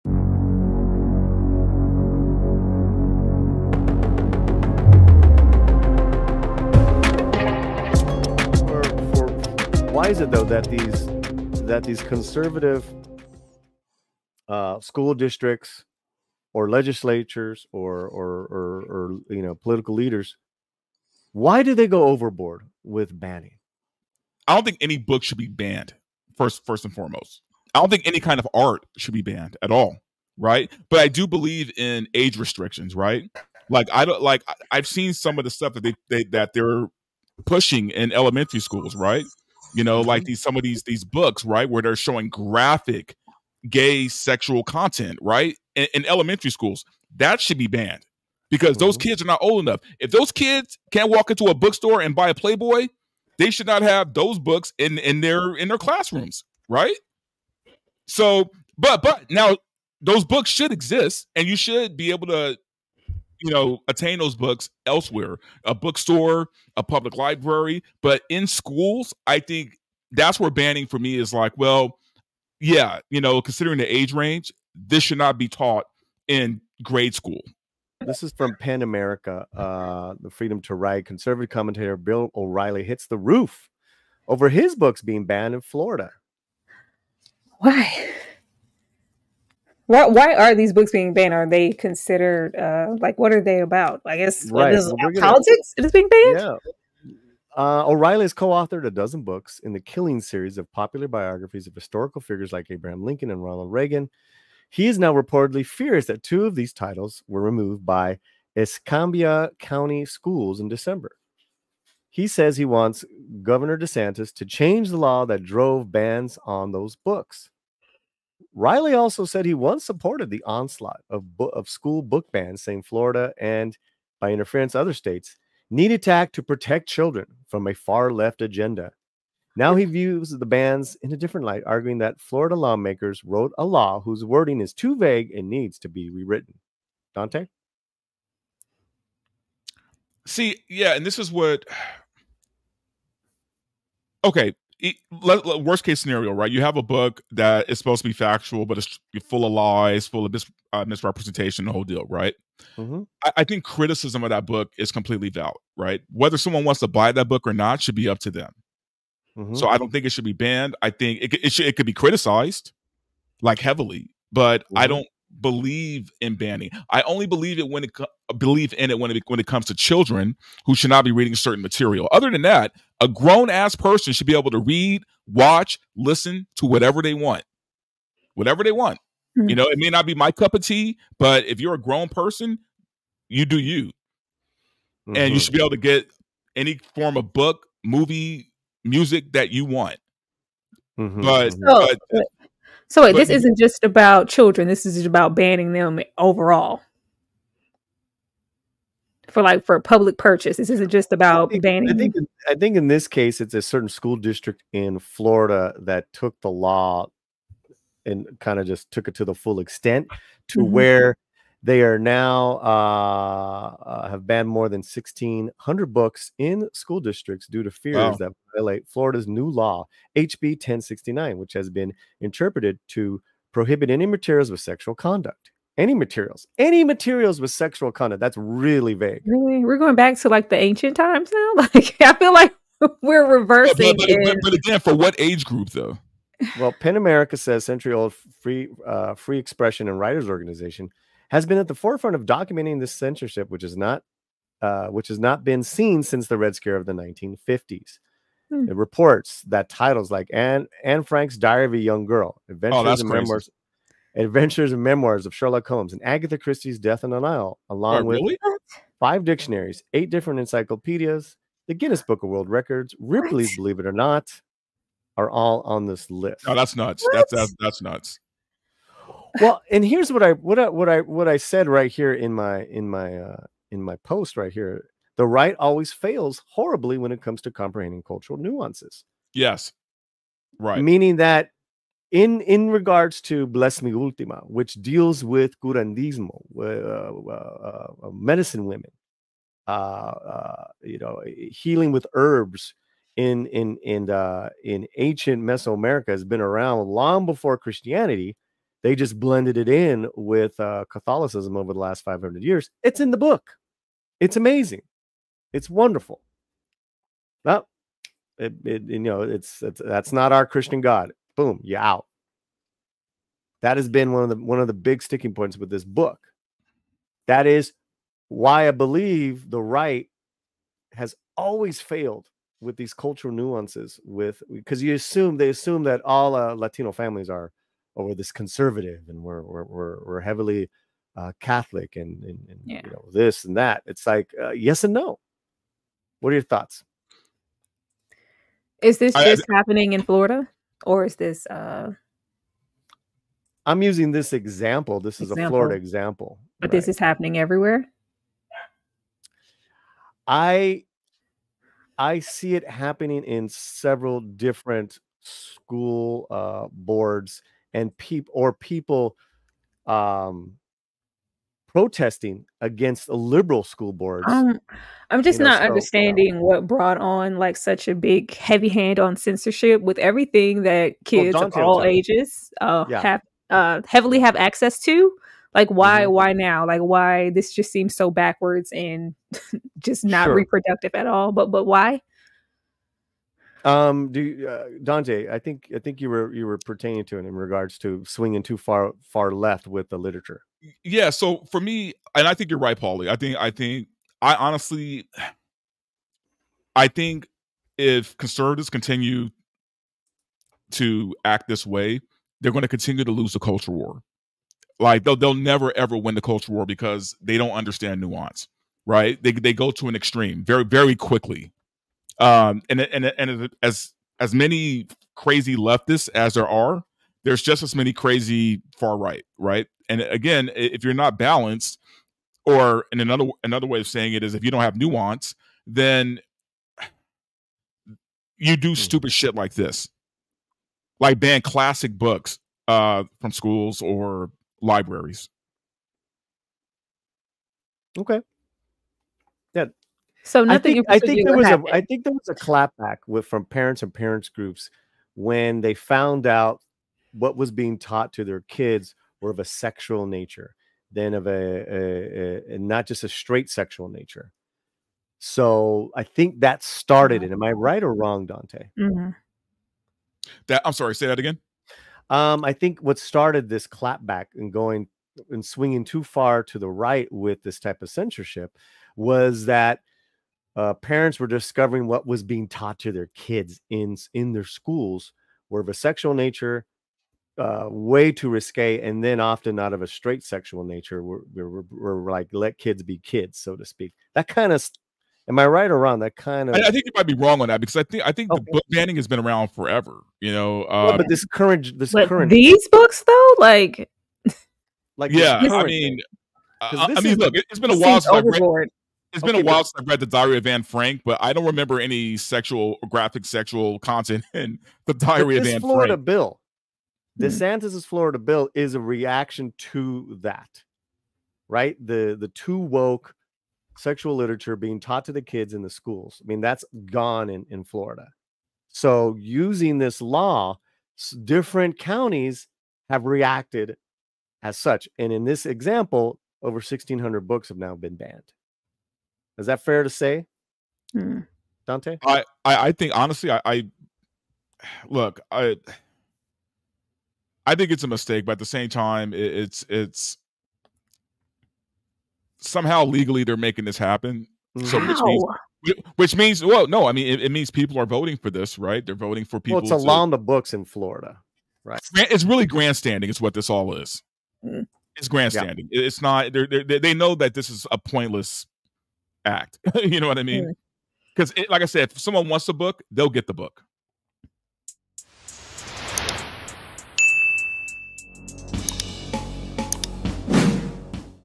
The RIGHT's Strange Fascination with Book Banning?
Paulie @StruggleReviewzTV @verbalriotshow tacle the touchy issues of conservative state to local legislatures banning books, including classic books to modern one, from the classroom.